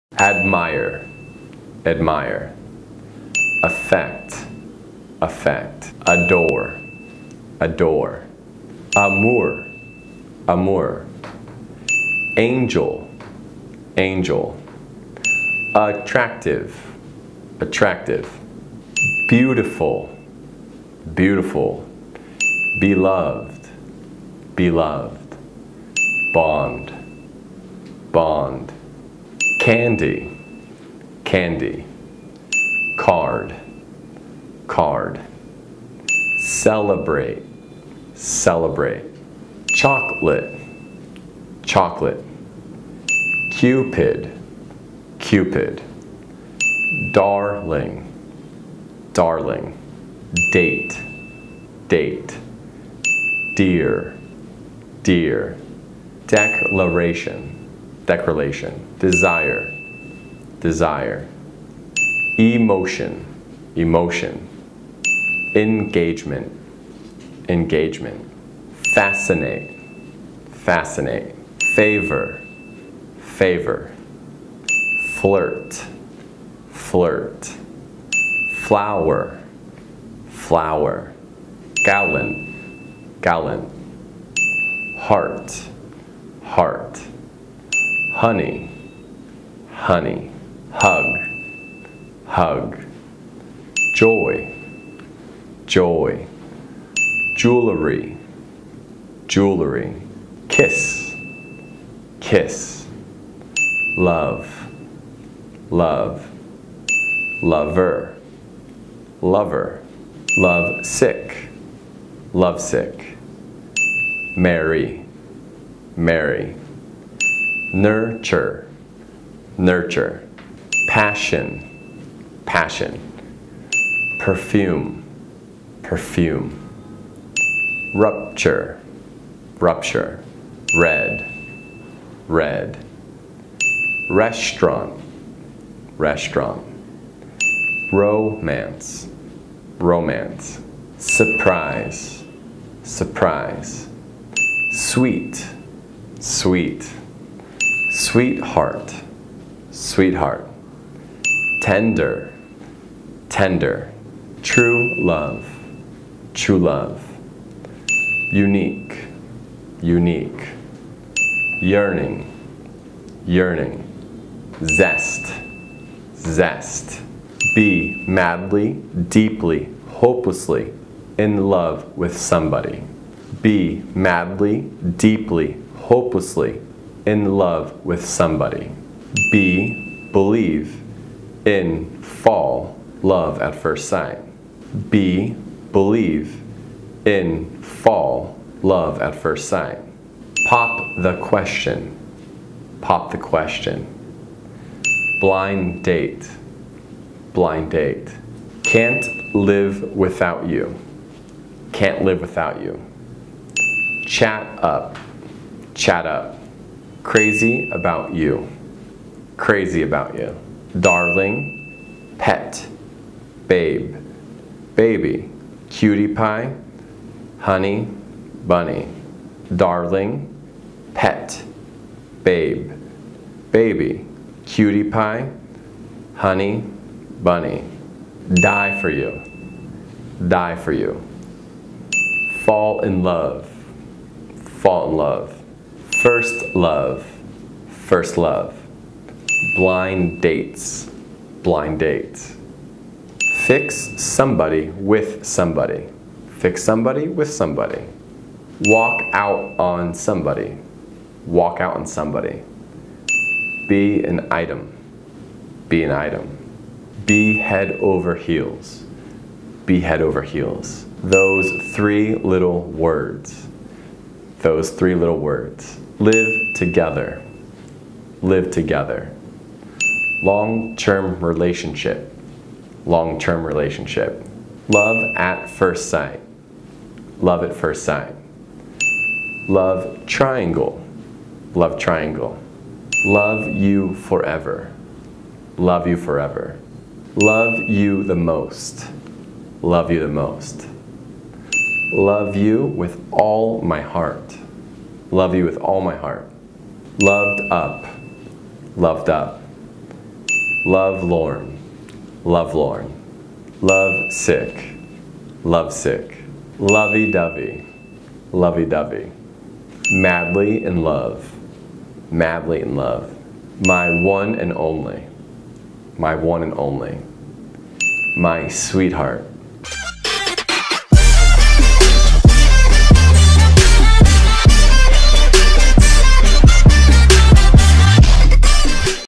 [English Town] Từ vựng tiếng Anh chủ đề TÌNH YÊU
Tình yêu luôn là một chủ đề thú vị dù là ở trong tiếng Việt hay tiếng Anh.

Bài học của hôm nay sẽ là bộ từ vựng tiếng Anh chủ đề TÌNH YÊU, hãy cùng học xem để nói về chủ đề này thì có những từ như thế nào nhé!

Đừng quên đón xem nhiều bài học bổ ích khác của thầy Dustin nhé!
🏠 ET 1: Lầu 4-5, Toà Nhà Sumikura 18H Cộng Hoà, QTB